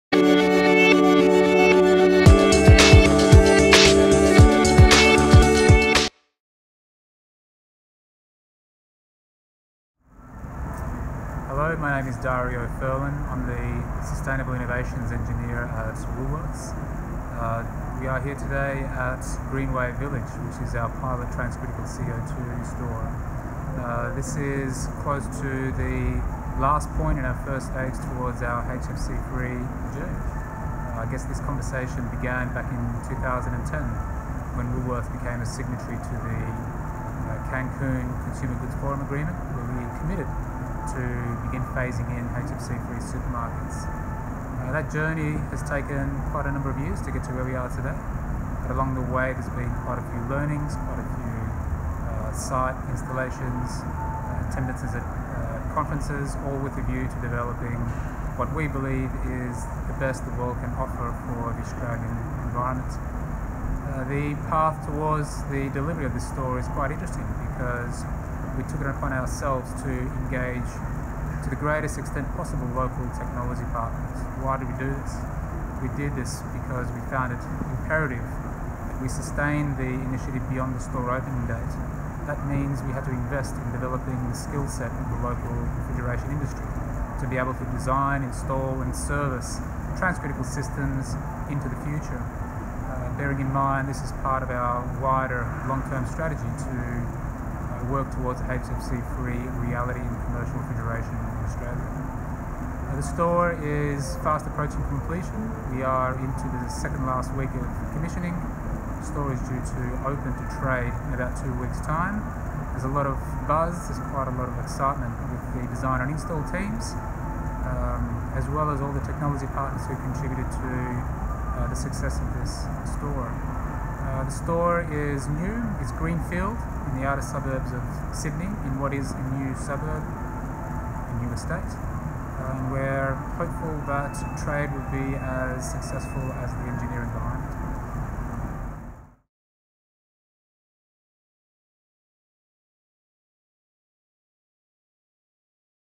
Woolworths’ first transcritical CO2 store up-skills local Australian refrigeration industry
Two-weeks before the opening of Woolworths’ pilot transcritical store, Dario Ferlin, Woolworths sustainable innovations engineer, discussed the retailer’s commitment to becoming HFC-free and the store’s significant role in up skilling Australia’s local refrigeration industry.